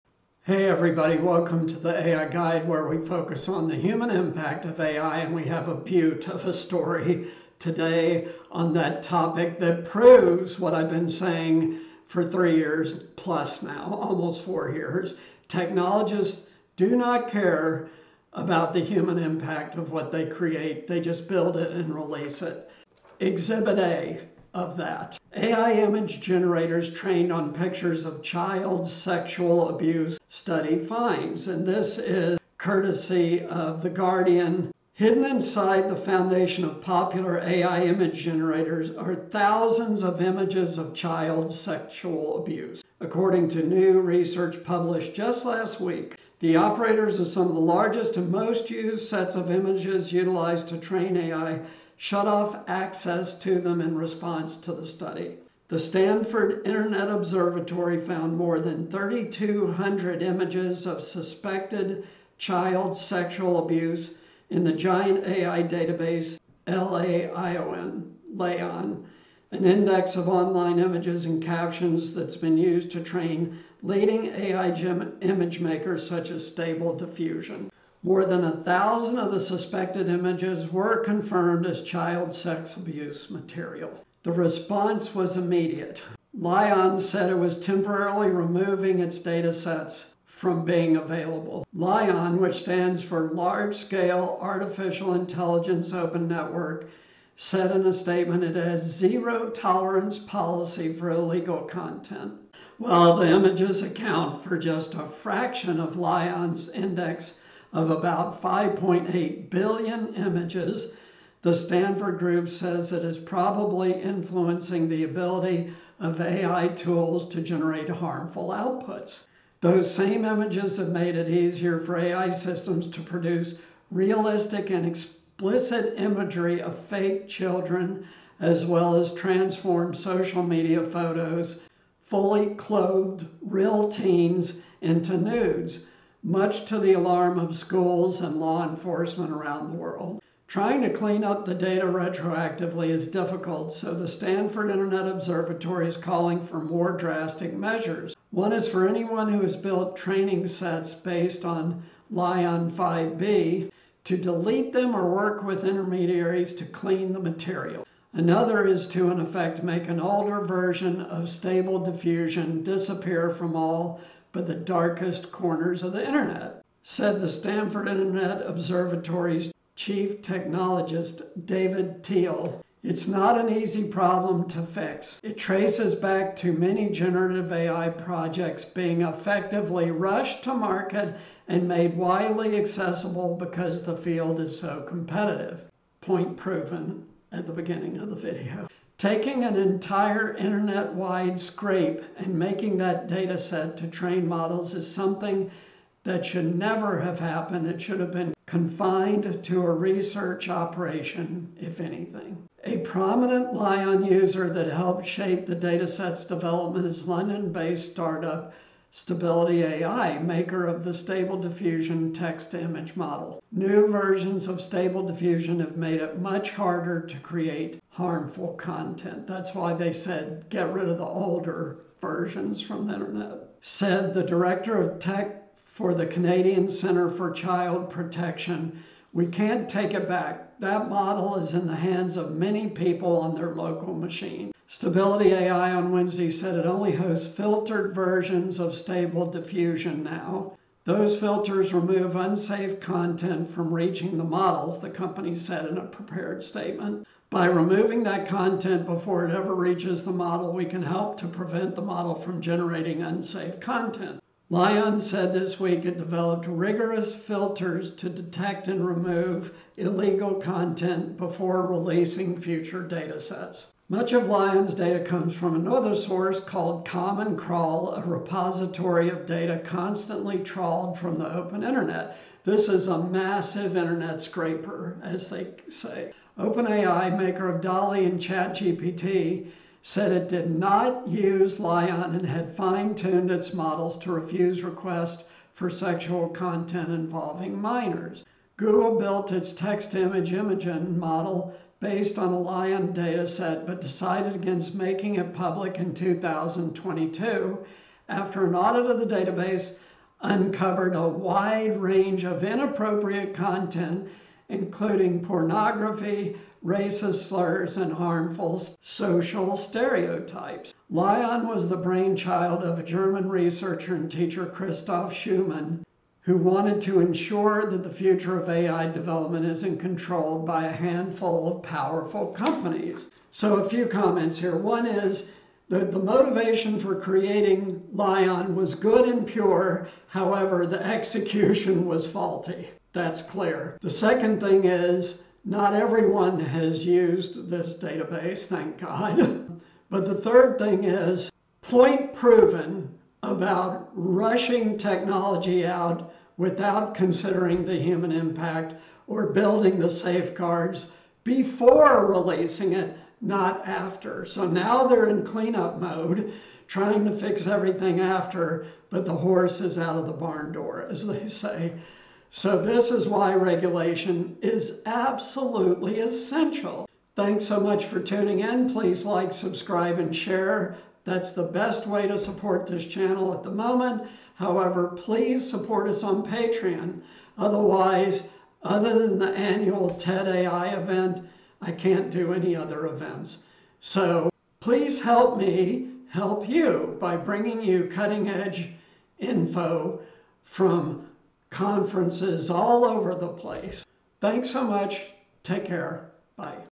🚨🤖 Urgent: Stanford Internet Observatory Uncovers Shocking Findings in LAION Database! 🌐🔍 [EP346]
Hello, AI community, and welcome back to The AI Guide channel. Today, we have some deeply concerning news to discuss. Stanford Internet Observatory has recently made a groundbreaking discovery, shedding light on a distressing issue within the AI landscape – the LAION database of images, riddled with child pornography. 🚫😢

🔥 What This Video Covers:

Critical Investigation: Join me as we delve into the details of Stanford Internet Observatory's investigation. We'll uncover how they stumbled upon the LAION database and the implications of its disturbing content. 🕵️‍♂️🔍

Nature of the Findings: Brace yourselves for an in-depth exploration of the shocking discoveries within the LAION database. Stanford's revelations about the presence of child pornography in an AI-related dataset demand urgent attention and discussion. 📉🚨

Ethical Concerns in AI: Let's discuss the broader ethical implications of this discovery. We'll explore the responsibilities of data collectors, the potential risks associated with AI datasets, and the urgent need for ethical guidelines in the rapidly evolving world of artificial intelligence. 🤝💻

Calls to Action: Discover what steps Stanford Internet Observatory is advocating for in response to these distressing findings. From transparency to improved oversight, we'll discuss the measures that can help prevent such instances in the future. 🌐🛡️

🚀 This video is not just a revelation but a call to action. It's crucial for us as a community to be aware, engaged, and proactive in addressing the ethical challenges that arise in the AI landscape.

Let's foster a dialogue on how we can collectively contribute to a safer and more responsible AI future. 🌟💙